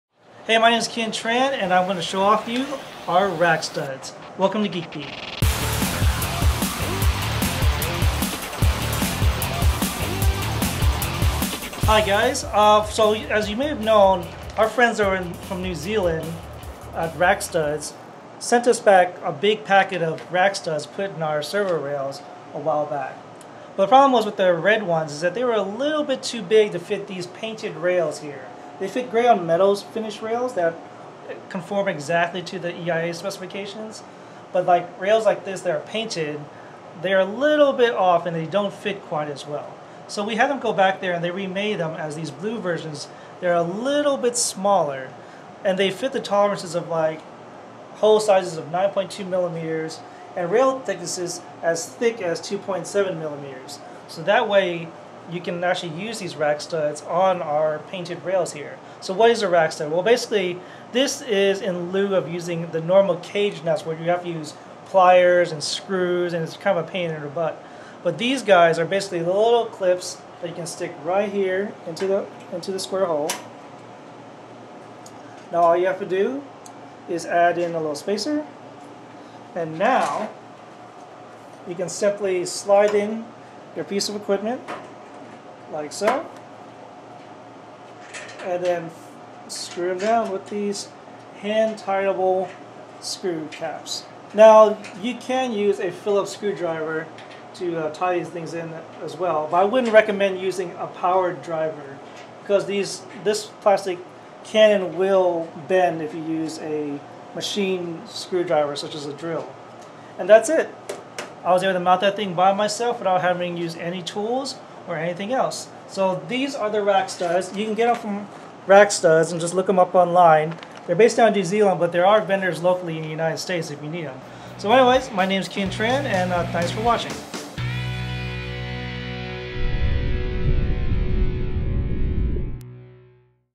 Review: Rackstuds - The Smart Rack Mounting System
Our engineer, Kien "The Kiengineer" Tran, shows you a very useful product from our friends at Rackstuds.